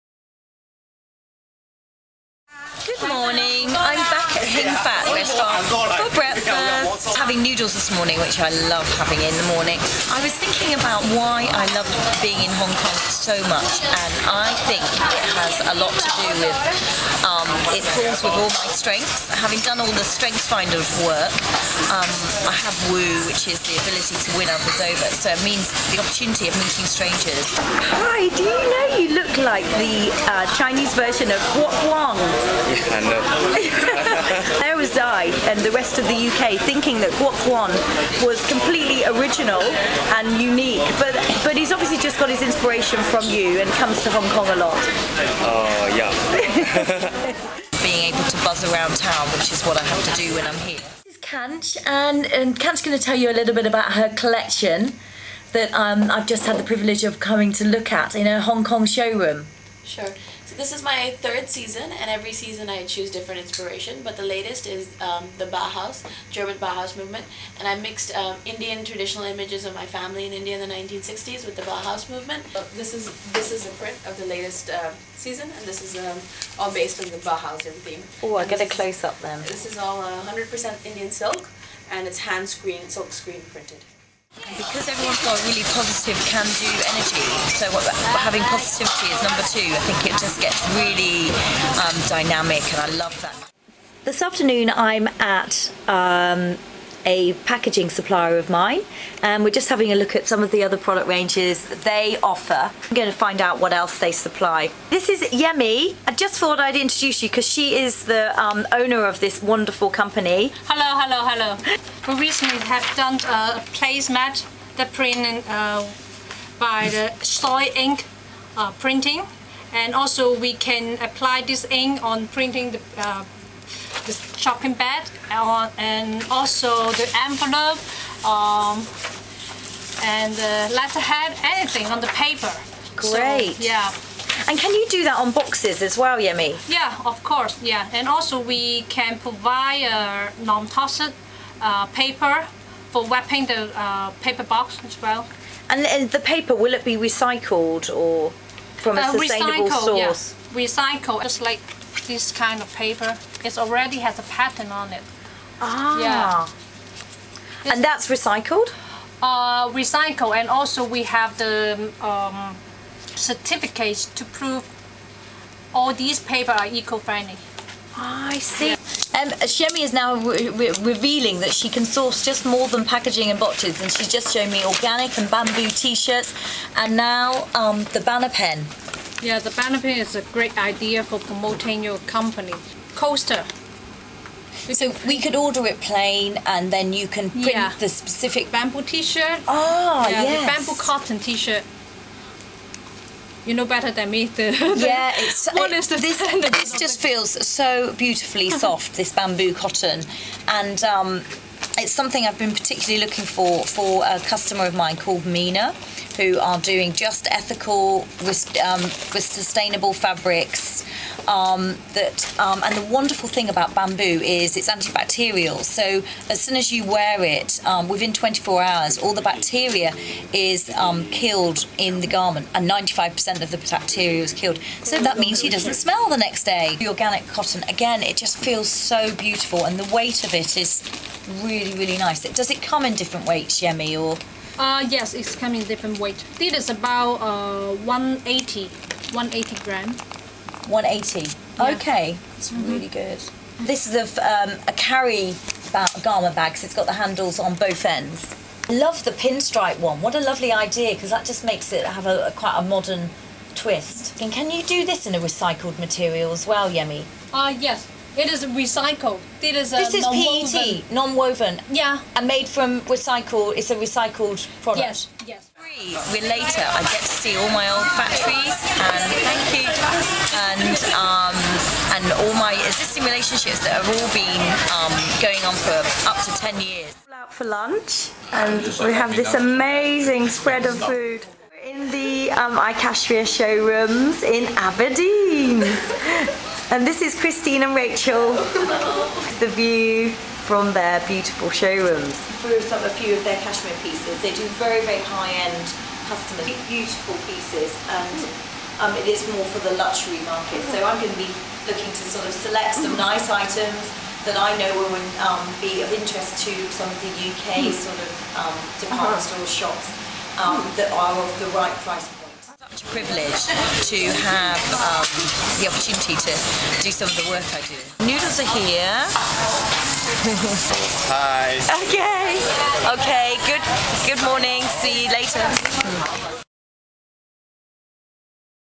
Show rooms in Hong Kong: Katie Young sourcing fashion
Katie Young Design.. recent trip to Hong Kong. Katie meets up with some designers, show rooms, factories and talks about why she loves Hong Kong and eco resources.